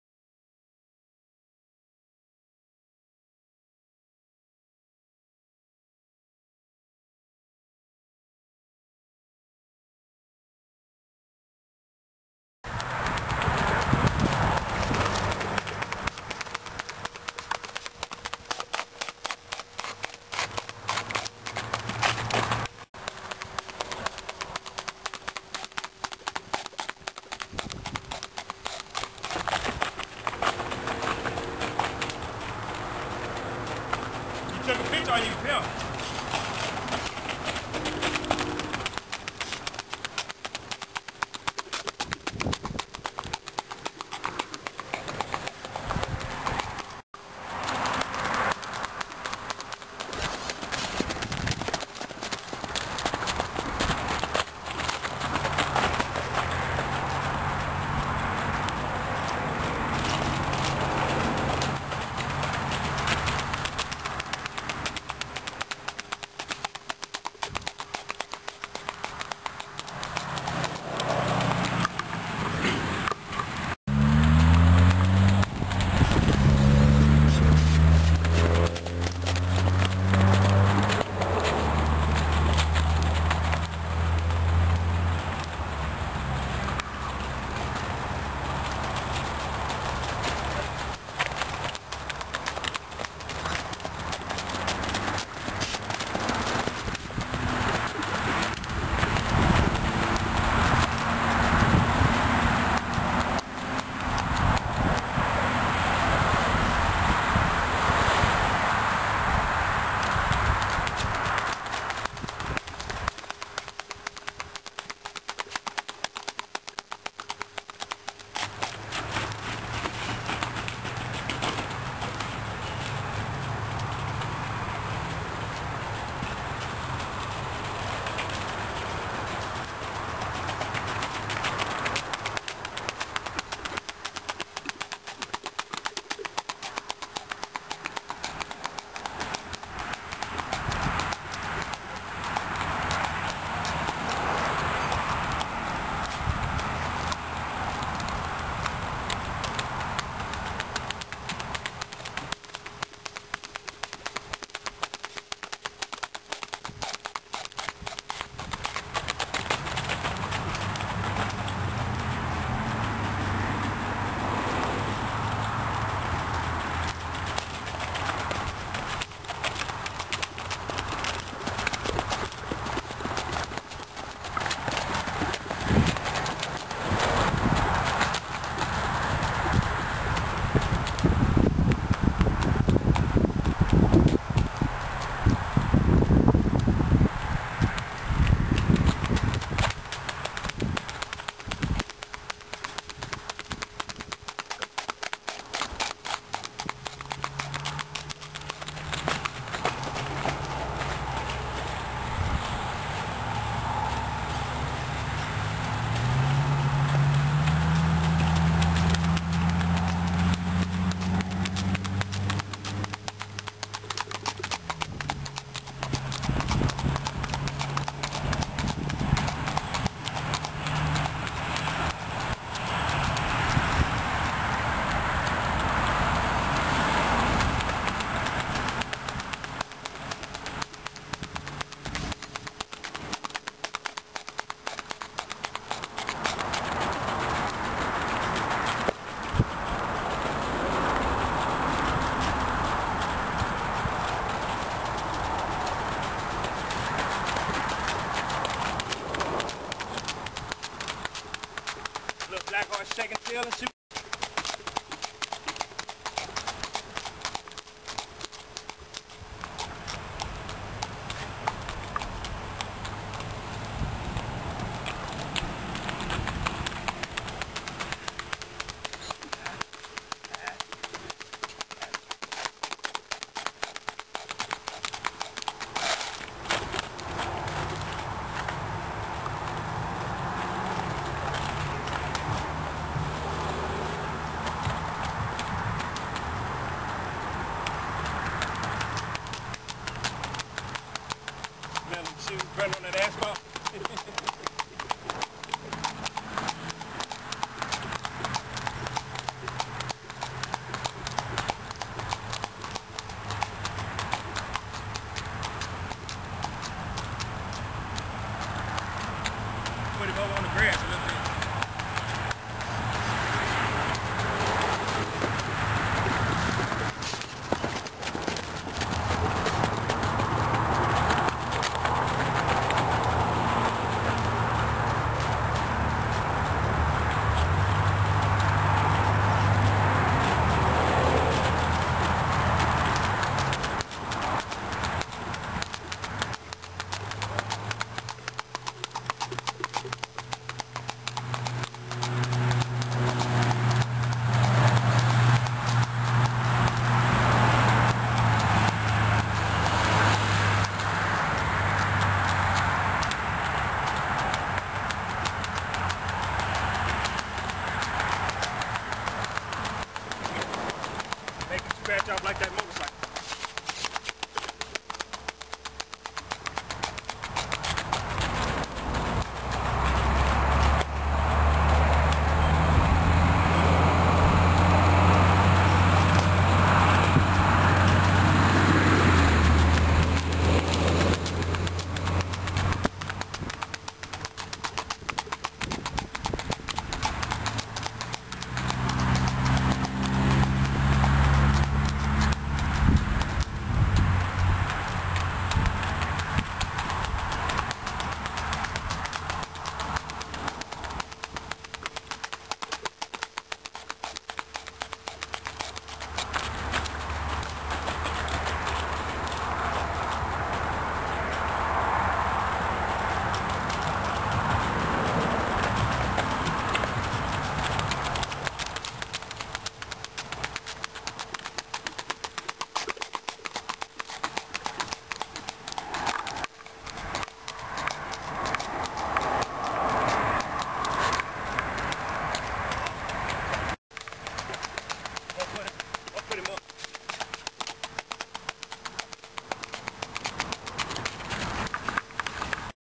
Uncle Pine's Son "aka" UPS Training #3
Uncle Pine's Son "aka" UPS. 5yo Singlefooting Stallion. He has plenty more to go but the ground was uneven and I'm not trying to hurt me or the horse.